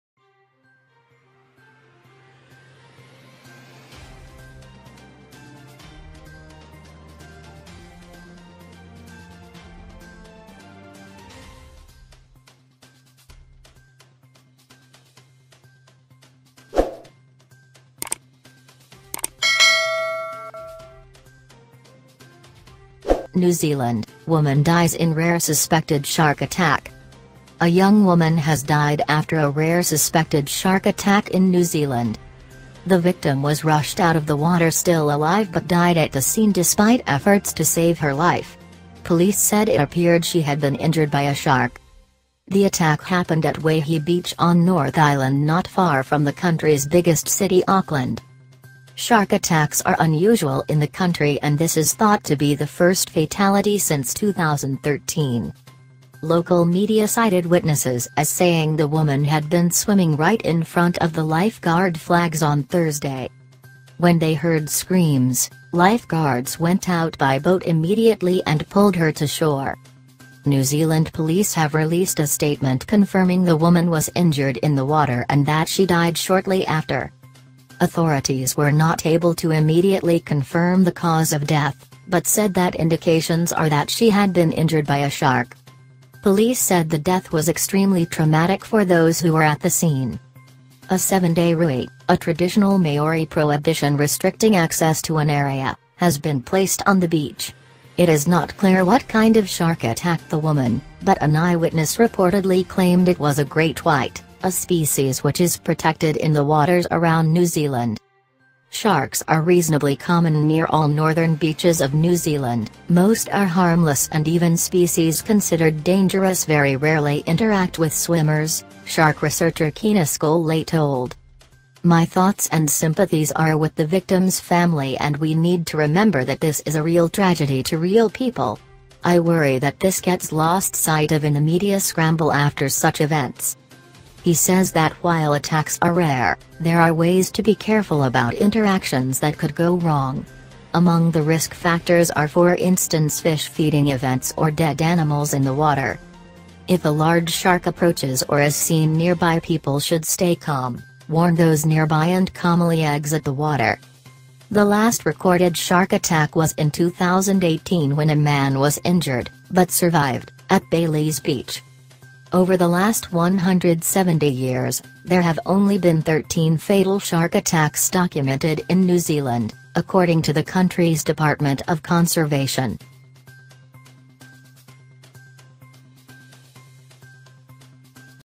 New Zealand: Woman dies in rare suspected shark attack I DECENT NEWS I
A young woman has died after a rare suspected shark attack in New Zealand.
The victim was rushed out of the water still alive but died at the scene despite efforts to save her life. Police said it appeared she had been injured by a shark.
The attack happened at Waihi Beach on North Island not far from the country's biggest city Auckland.
Shark attacks are unusual in the country and this is thought to be the first fatality since 2013.